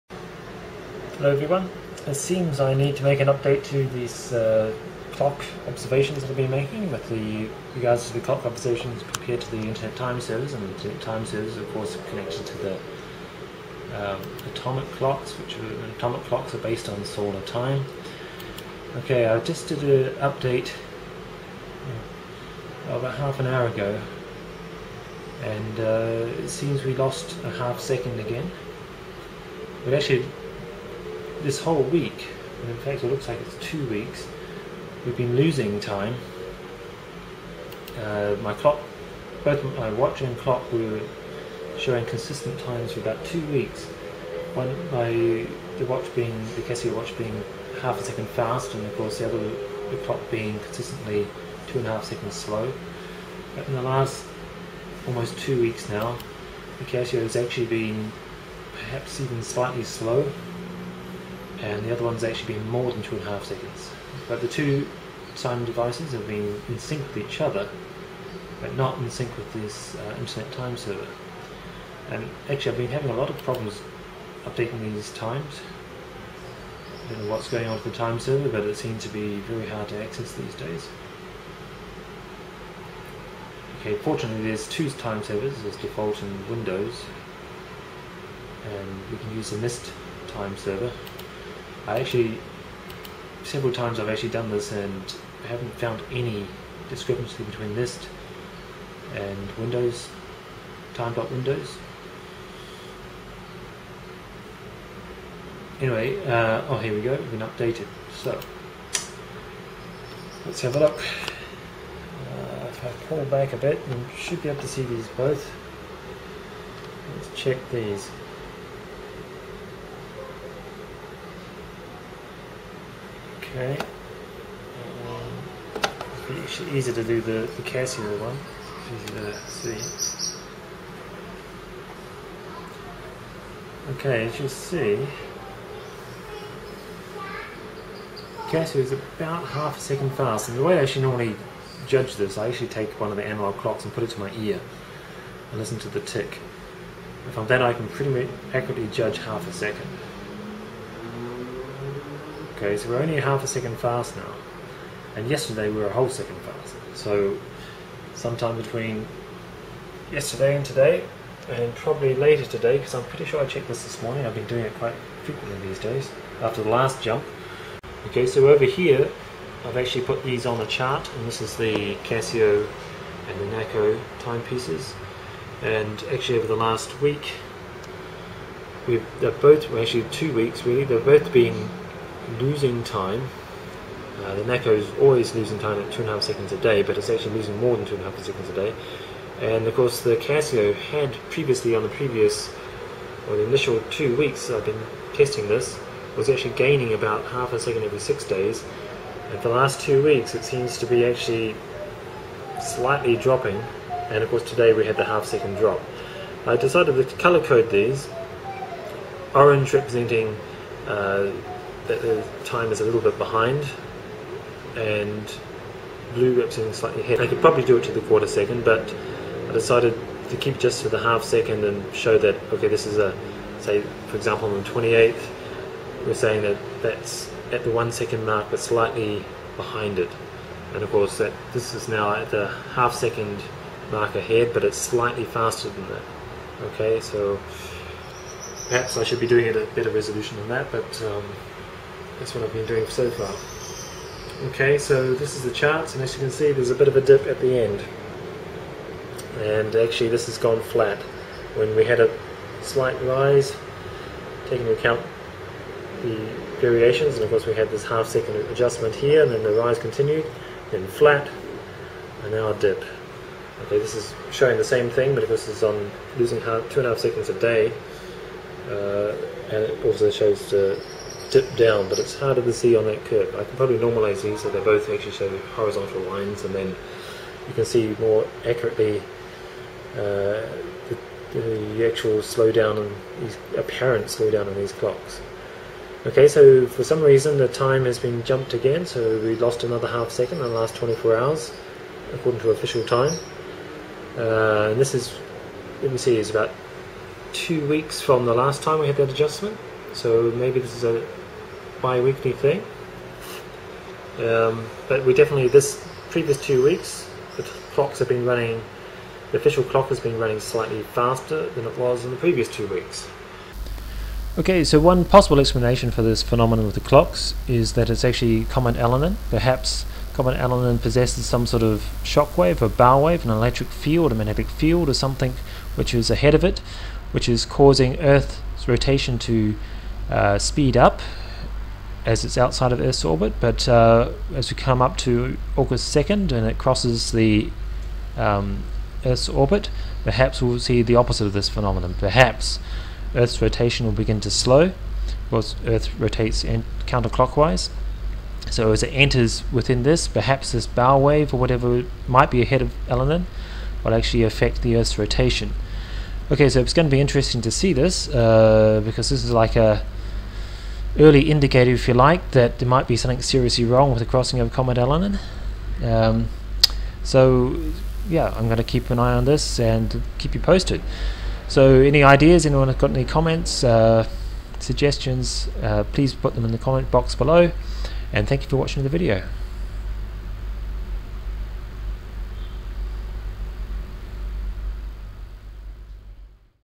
Clocks vs Comet Elenin - We just lost another half second
Today we had another half second jump in the official time. This is after 2 weeks of a general slight daily shortening of the day when compared to the previous 2 weeks.

Too early to say that this is related to the approach of comet Elenin but also can't rule it out either.

Perhaps on August 2nd when comet Elenin crosses Earth's orbit around the Sun, we'll see a reversal of this trend ie. a slow down in the official clock time to match a slowdown in Earth's rotation. Let's hope not but it is something to keep an eye on.

This little experiment began in response to this AP story on Yahoo News.

"AP EXCLUSIVE: Power grid change may disrupt clocks"

"WASHINGTON -- A yearlong experiment with the nation's electric grid could mess up traffic lights, security systems and some computers — and make plug-in clocks and appliances like programmable coffeemakers run up to 20 minutes fast."